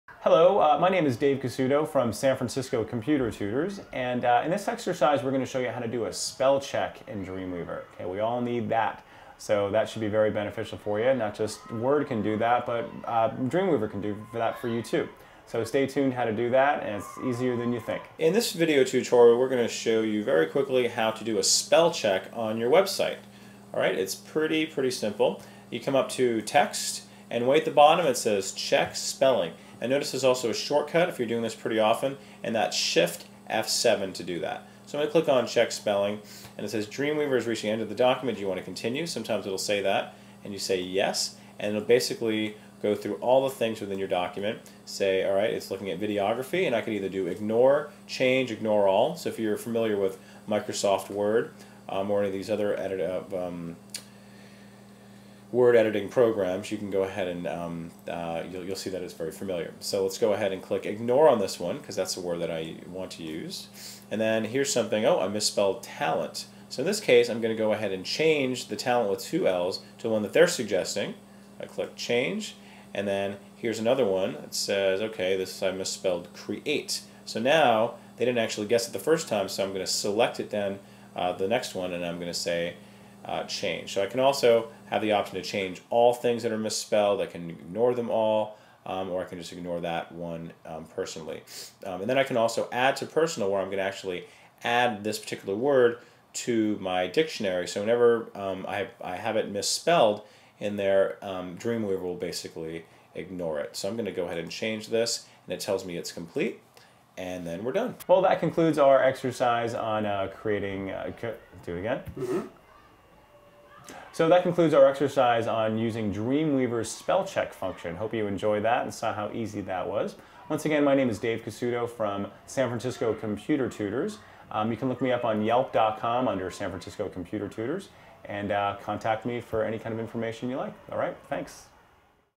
Dreamweaver Tutorial : How to Spell-Check a Web Page With Dreamweaver
Spell-checking a Web page using Dreamweaver can by done by clicking on "Text" and finding the "Check Spelling button" or simply holding down the shift key with the F7 key as a shortcut. Check the spelling on a Web page with instructions from a certified Dreamweaver tutor in this free video on Web design.

Expert: Dave Casuto
Bio: Dave Casuto is a certified Dreamweaver tutor working in San Francisco, Calif.